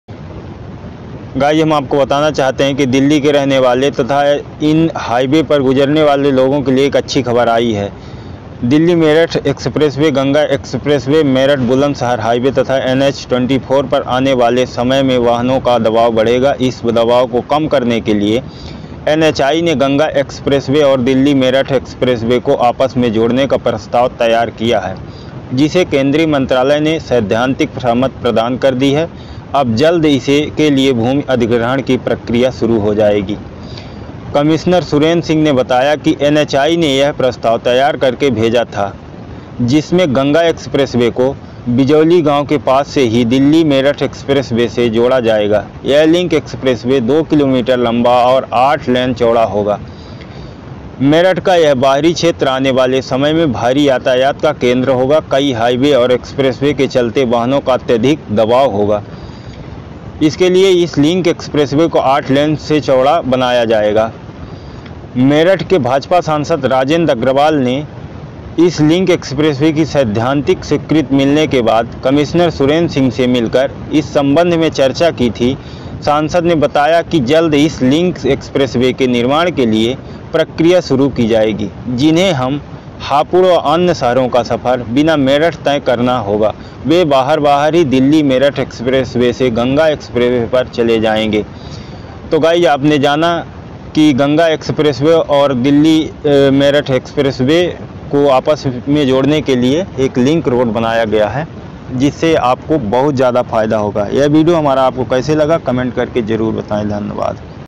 Delhi to merath or ganga expressway//one link road// न्यू प्रोजेक्ट अब दिल्ली में बनेगा लिंक रोड//
Ghaziabad: The National Highways Authority of India (NHAI) has decided to construct an arm of the Delhi-Meerut Expressway that will link Modinagar in Ghaziabad with NH-235 in Meerut. The connector loop of the country’s widest expressway will eventually be extended up to the Ganga Expressway, India’s longest that is under construction. The announcement comes days after all four phases of the DME were opened for vehicular traffic. Officials are calling the 14km offshoot the 5th phase of the DME project. It will branch out from the Dasna to Meerut section of the expressway at Zainuddinpur village near Modinagar. Once ready, the expressway will give travellers a seamless access to eastern UP districts through the Ganga Expressway.
“The connector will be 14km long and will link DME with NH-235.